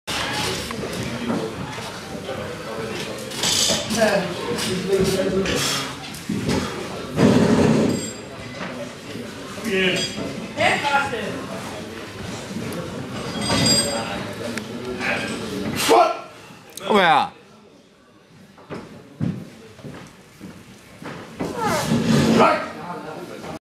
Johannes W. Andersen - AaSK Open 2014 - 230 kg squat - 1. attempt - miss
Not deep enough